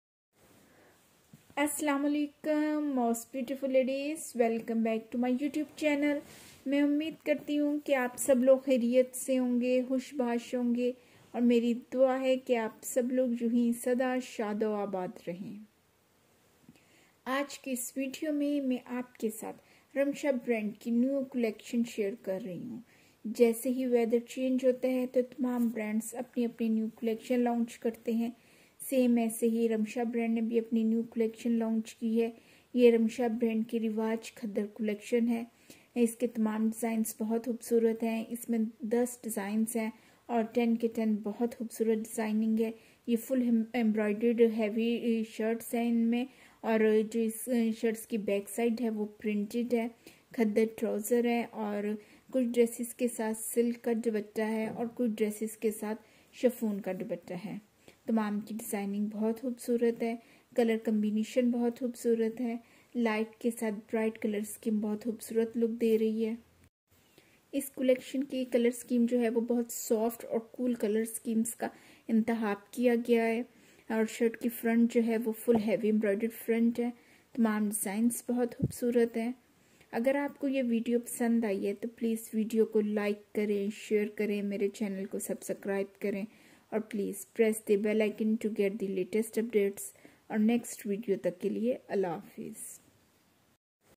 Riwaj Khaddar Collection 2022/ Embroidered Khaddar Collection 2023/ Wardrobe Essentials
Assalam-o-Alikum My YouTube Family...

Here I share with you Ramsha New  "RIWAJ Winter Collection 2022" .
This is a Heavy Embroidered 3Pc Khaddar Collection with Digital Printed Silk Dupatta.